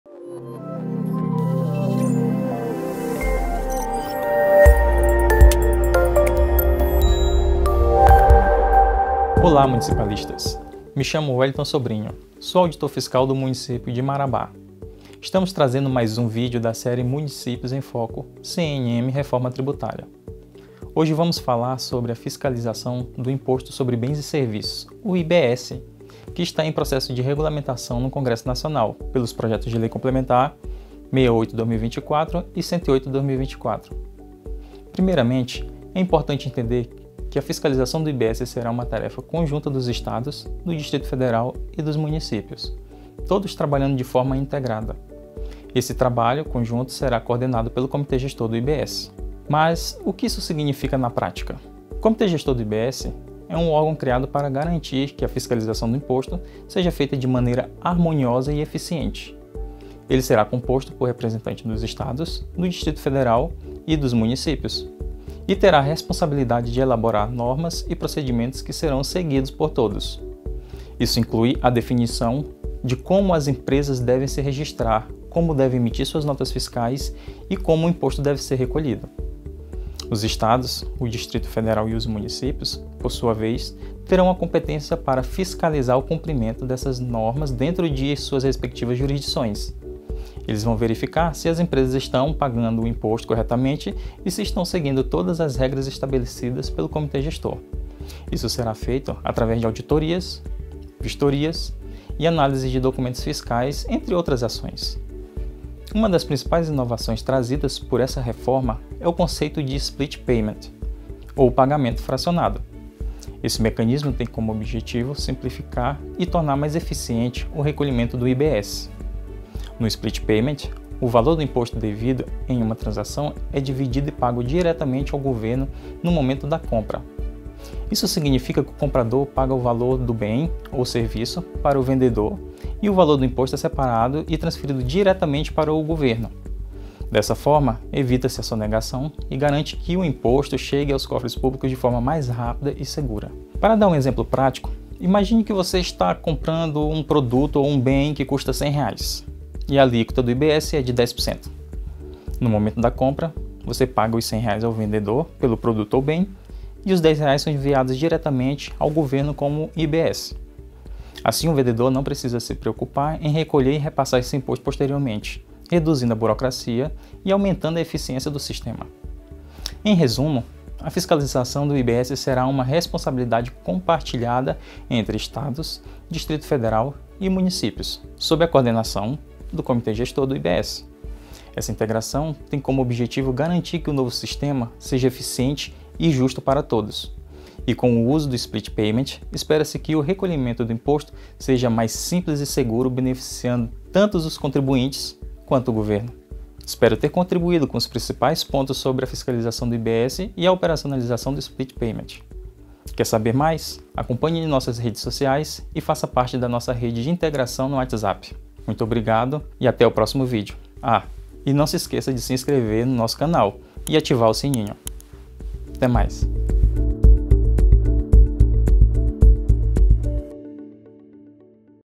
Municípios em Foco | Episódio 26: Fiscalização e Split Payment
Novo conjunto de vídeos da Confederação Nacional de Municípios (CNM) começam a ser publicados e trazem informações sobre a Reforma Tributária e o impacto das mudanças nos Entes federados.

Confira!